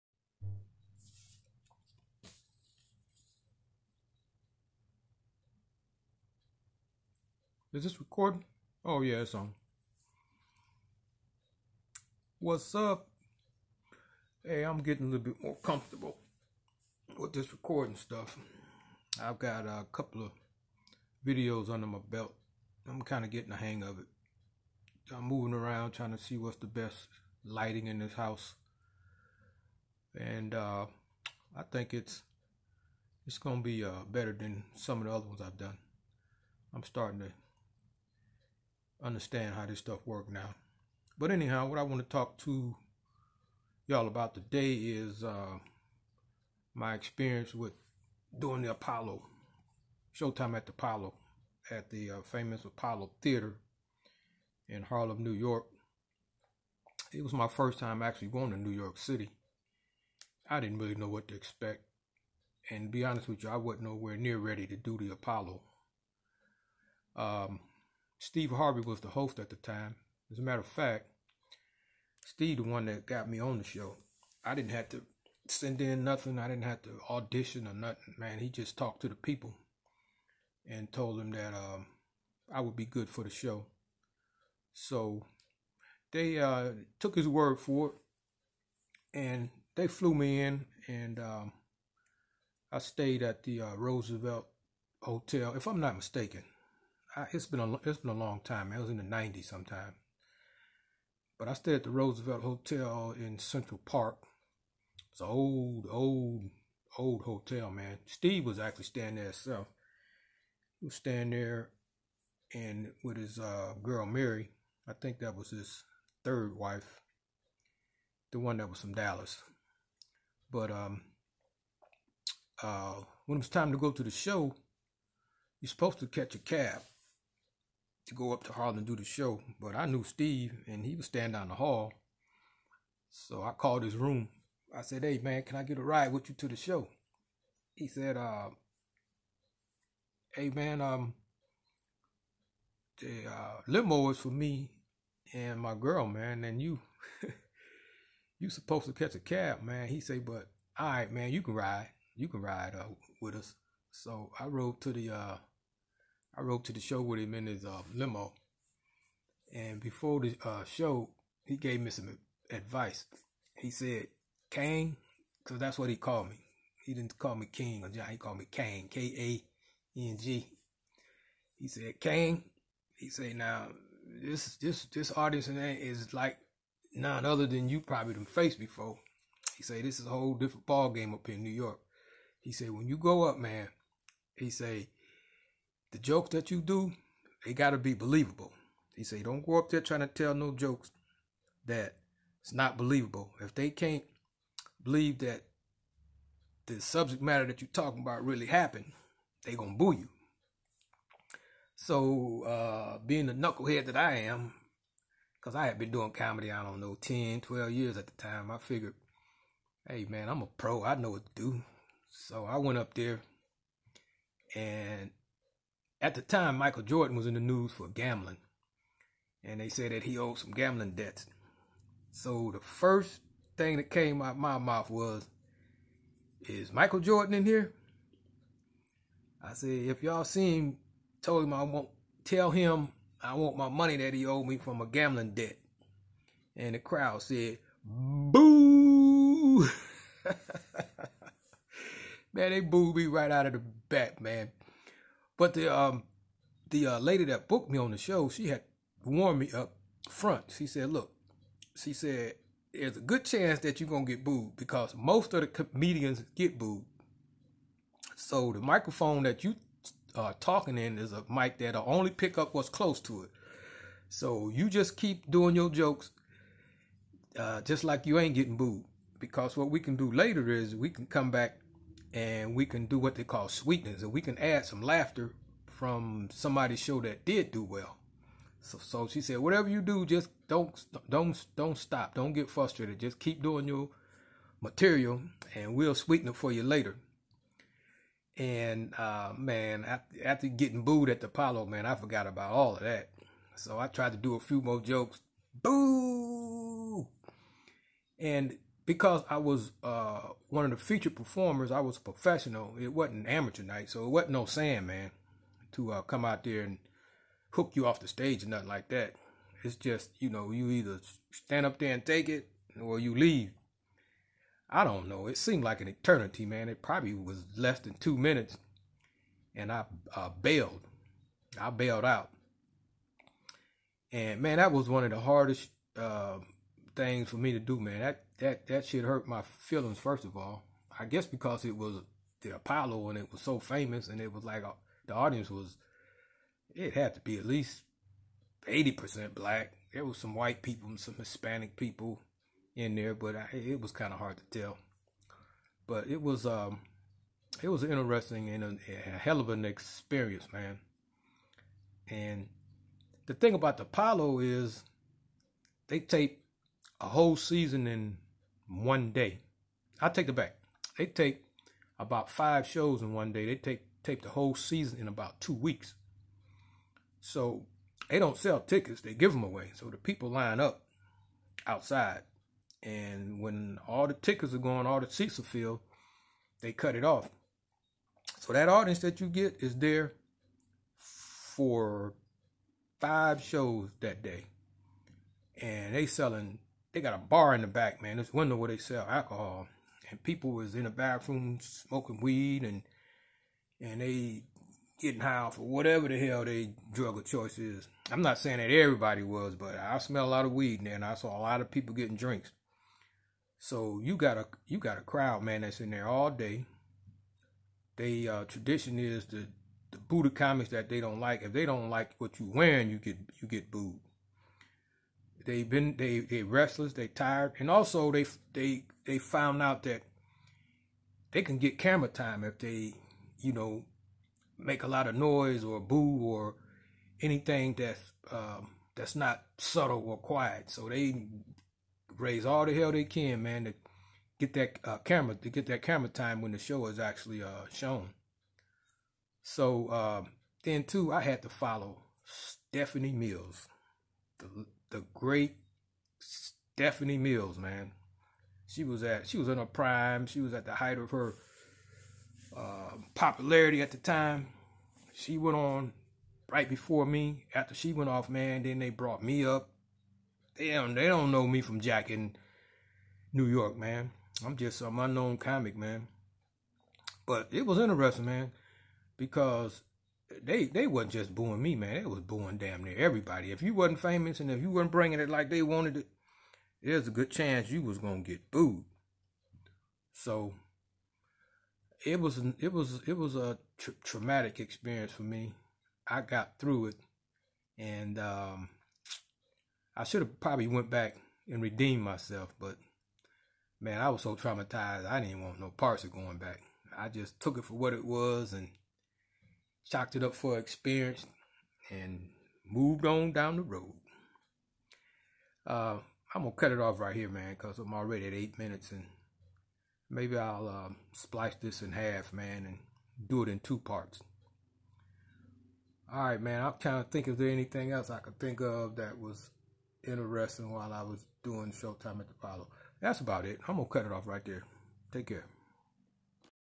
Show Time At The Apollo ended up being almost no time at the Apollo.
A story about one of my T.V. experiences in the Big Apple in the 90's. comedian...THE REAL JOHN KING